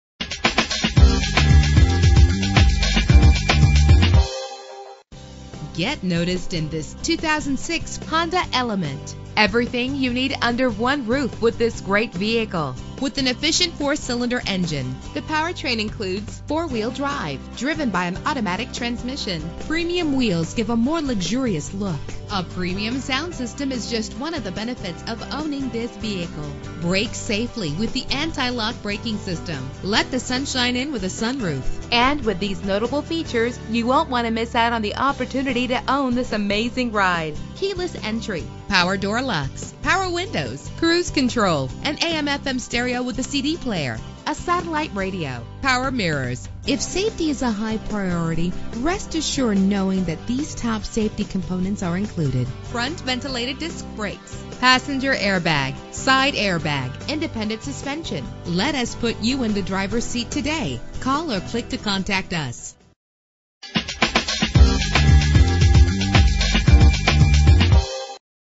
2006 Honda Element - Tallahassee FL
Year: 2006
Make: Honda
Model: Element
Trim: EX
Engine: 2.4 liter inline 4 cylinder 16 valve
Transmission: 4-Speed Automatic
Color: Alabaster Silver Metallic
Mileage: 112178
Address:
3963 W. Tennessee Street